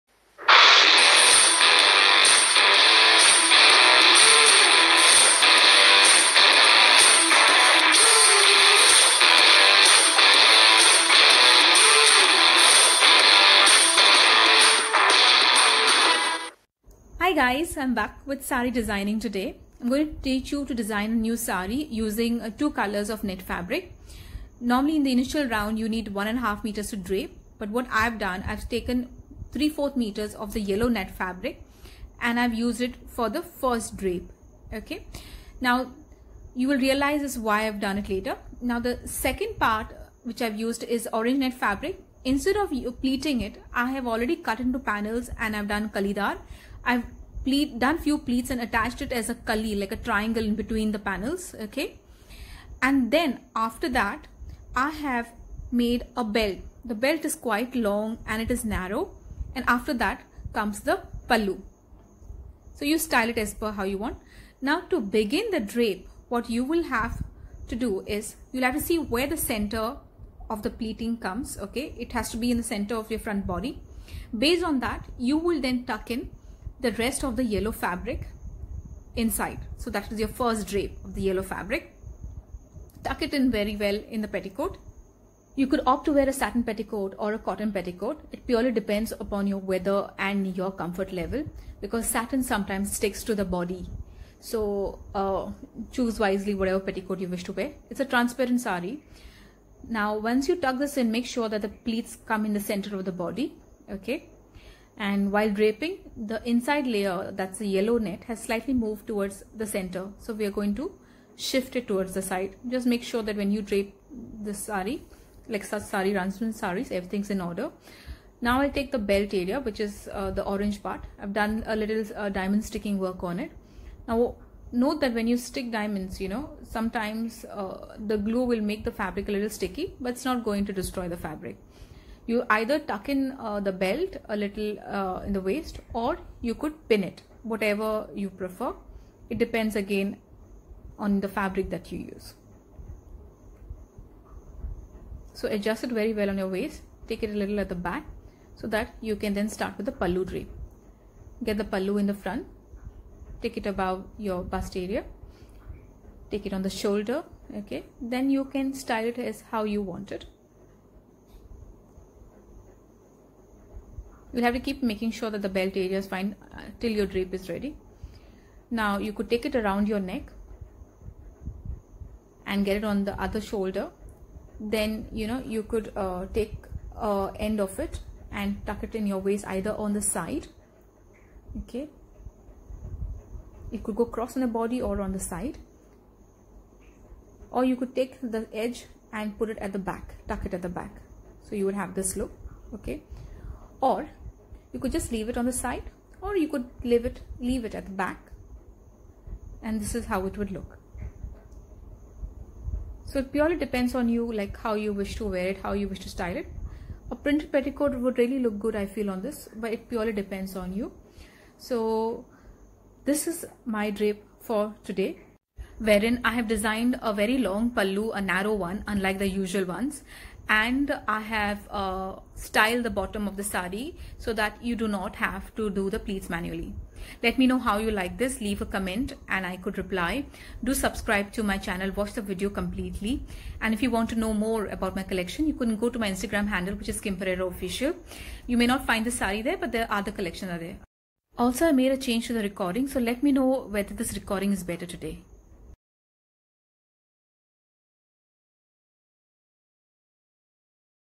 How To Design Your Saree | Saree Styles | DIY
Taken two colors of soft net fabric here.

* The first drape area fabric is around 30 inches long
* The orange drape area I have cut the fabric in panels like how you cut for anarkalis
* I attached a ready pleats section in between the panels for proper movement
* the belt area is narrow and you could tuck it in the petticoat or pin it
* the pallu is also long and narrow

You could opt for a saying petticoat since net is transparent. Even a printed cotton petticoat would look lovely in this plain saree